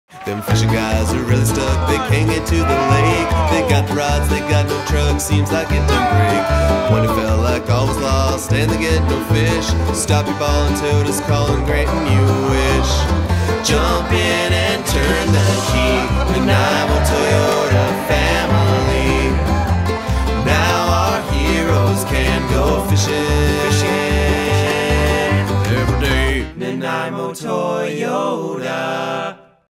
Nanaimo Toyota ft. TMATFR
The fellas run into some bad luck with their truck, but Nanaimo Toyota is there to save the day!